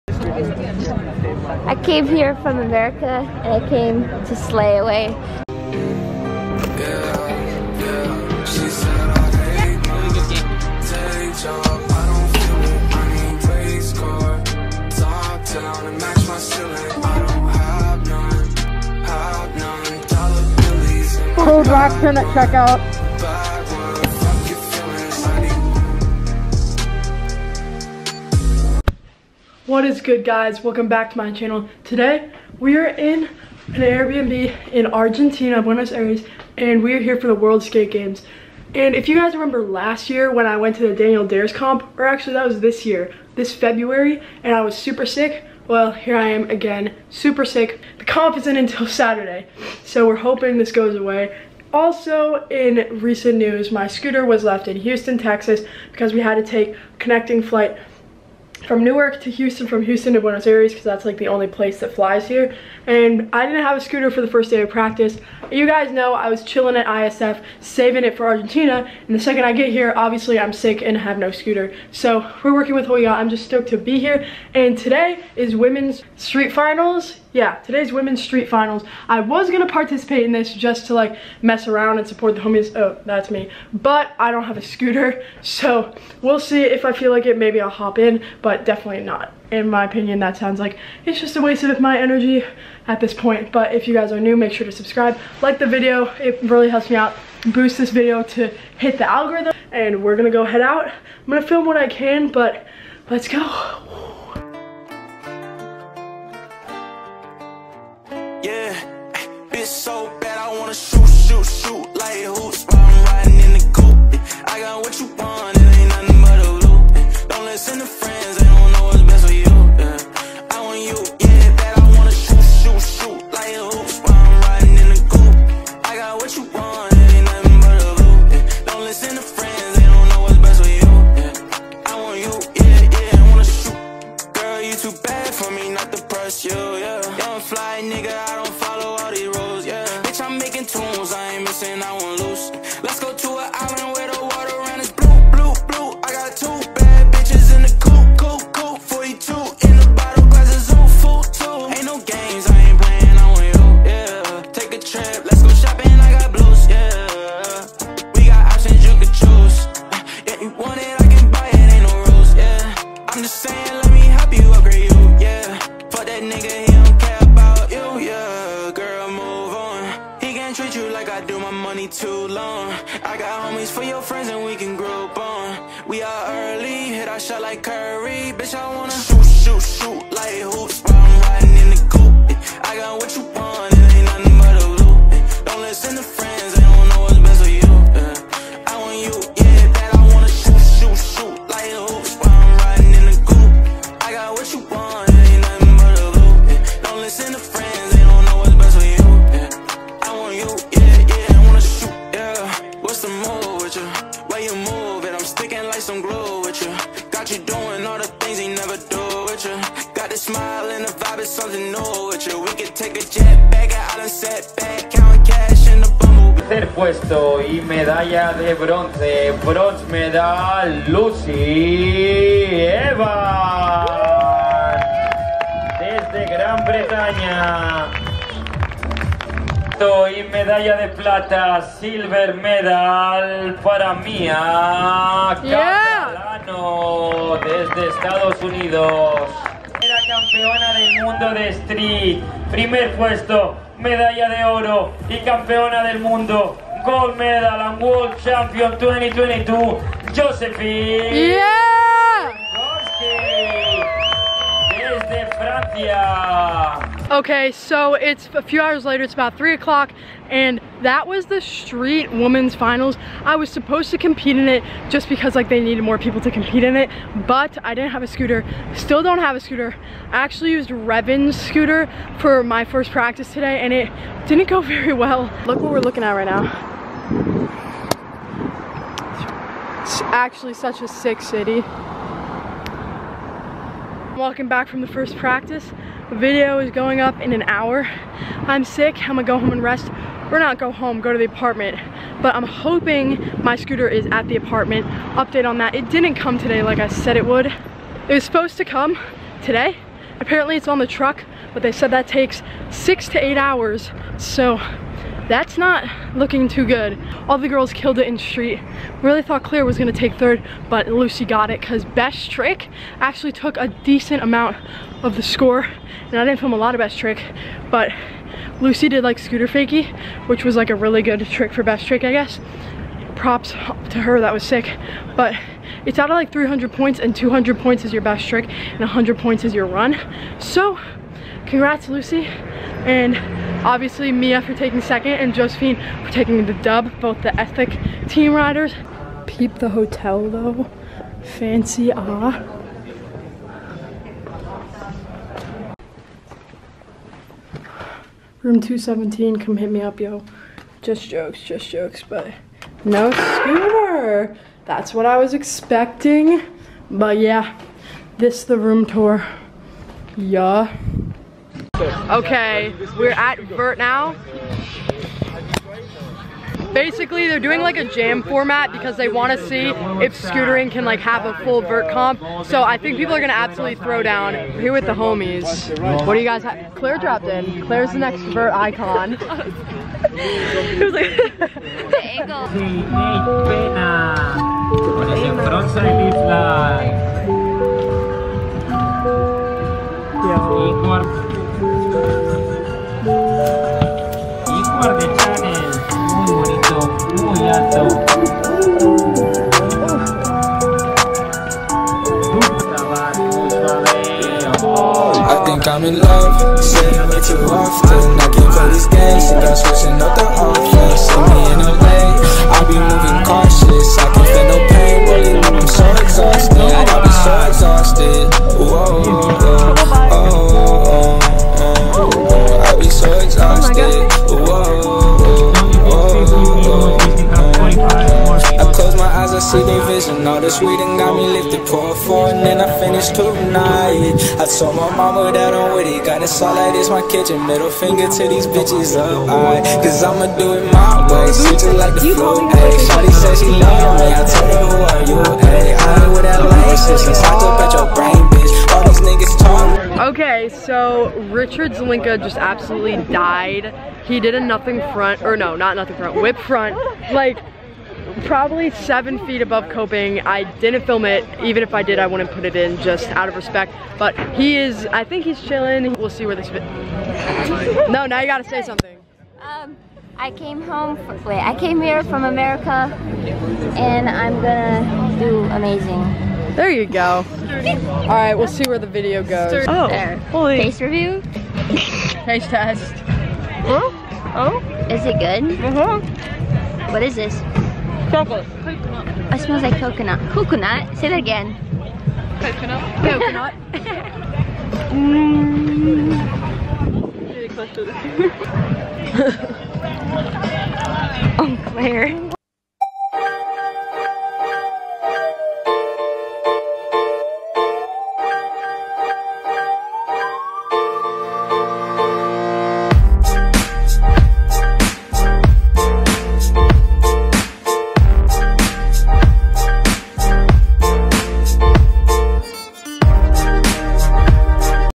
FIRST EVER WOMENS STREET WORLD FINAL! (world skate games)
first video from Argentina! its so sick here to be honest. so grateful to be here.
Videos twice a week!

Cameras I use:
Canon G7X